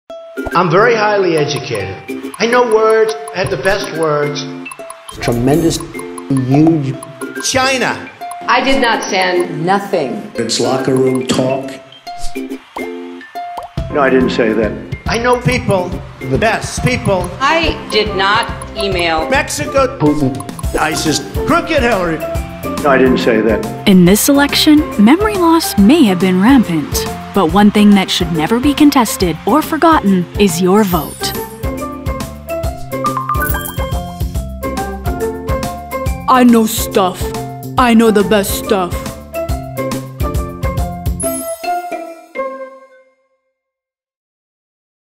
SettleMint BallotBox: a secure and immutable voting solution
SettleMint’s BallotBox application is a powerful demonstration of how technology can be used to ensure that your vote will never be forgotten, will never be disclosed, will forever remain secure and that the outcomes of an election can never be contested.